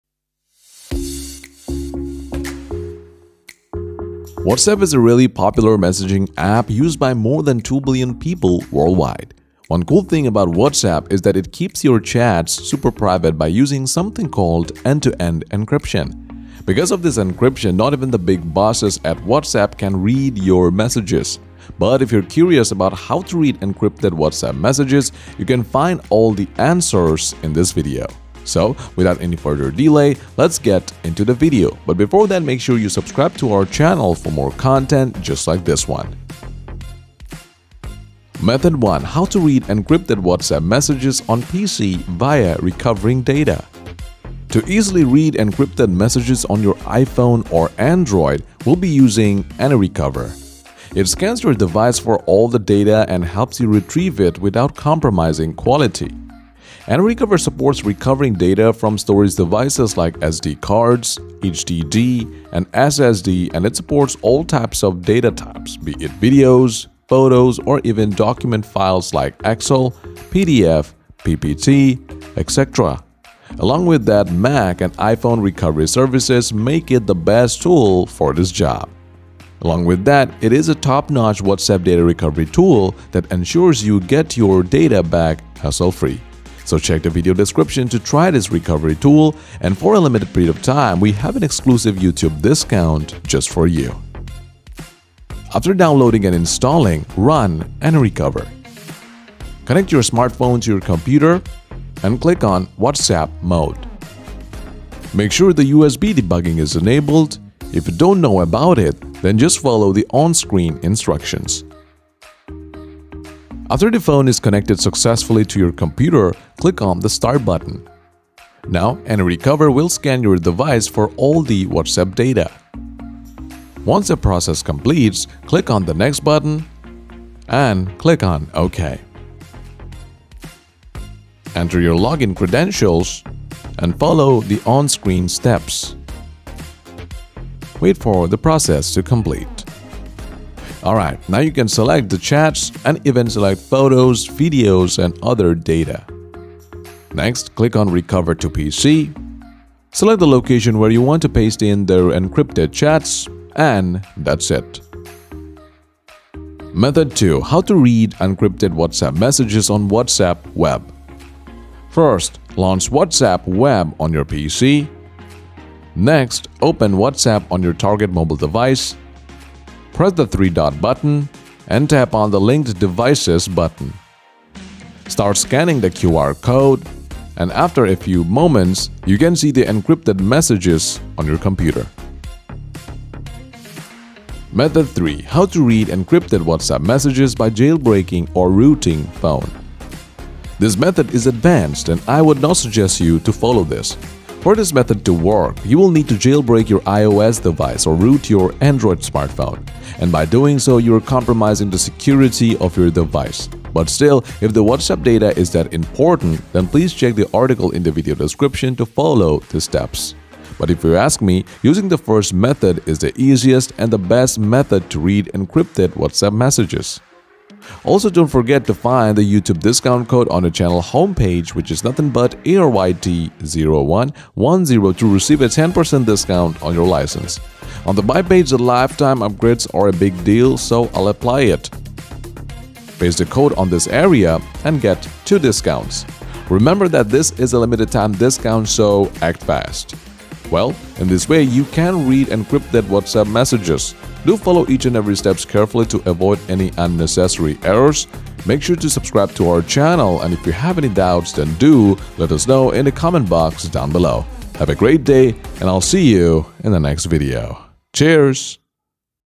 [2024 Updated] How to Read Encrypted Messages in WhatsApp
This video will show you how to read encrypted WhatsApp messages in 3 reliable ways, Read Encrypted WhatsApp Messages via AnyRecover 🔥

Here we offer 3 Proven Ways to read encrypted WhatsApp messages. Let's Dive into it!!

Timestamps:
00:00 Intro
01:31 Method 1: Read Encrypted WhatsApp Messages on PC via Recovering Data
02:38 Method 2: Read Encrypted Whatsapp Messages on WhatsApp Web
03:06 Method 3: Read Encrypted WhatsApp Messages by Jailbreaking or Rooting Phone

✅Method 1: Read Encrypted WhatsApp Messages on PC via Recovering Data
This way helps you easily read encrypted messages on your iPhone or Android
1. Launch AnyRecover after it has been installed
2. Connect your smartphone to your computer and click on “WhatsApp” Mode. Enable debugging.
3. After scan, enter your login credentials and follow the on-screen steps.
4. Recover to pc. Now it’s done!

✅Method 2: Read Encrypted Whatsapp Messages on WhatsApp Web
1. Launch WhatsApp web on PC.
2. Open WhatsApp on your target mobile device.
3. Press the 3-dot button, choose the “Linked devices”.
4. Scan the QR code and you can see the encrypted messages on your computer.

✅Method 3: Read Encrypted WhatsApp Messages by Jailbreaking or Rooting Phone
I don't recommend this method. For it to work you’ll need to jailbreak your iOS device or Root your Android phone, which compromise the security of your device. If the WhatsApp data is that important, then please check the TEXT GUIDE below to follow the steps

🟢TEXT GUIDE
How to Read Encrypted WhatsApp Messages [2023]

These ways are step-by-step guidance, if this video helps, pls